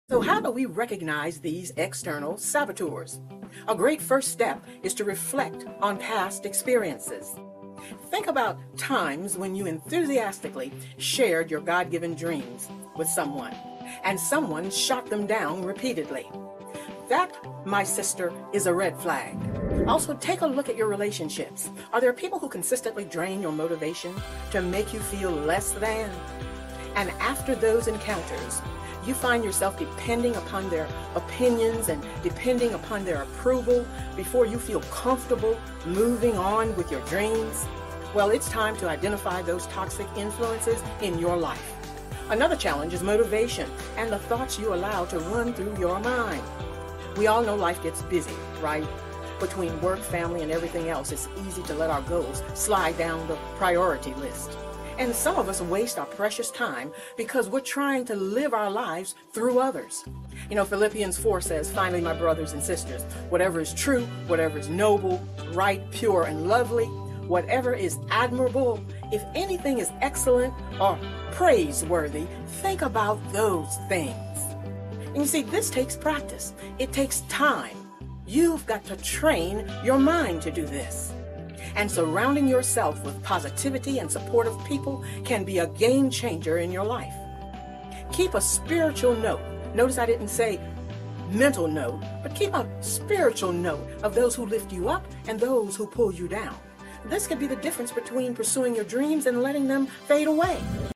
💕 W.O.I. | How to Spot Red Flags | Motivation | #4 | Personal Development | Personal Growth
This video is particularly tailored for women navigating friendships, family ties, and professional networks. Understanding the dynamics of these roles can empower you to make better choices for your mental and emotional well-being. Learn how to set boundaries, foster positive connections, and eliminate toxic influences from your life.

MAILING ADDRESS:
Dr. Hester Speaks, Llc (Charleston, SC)
Life Coach, Speaker, Educator, Author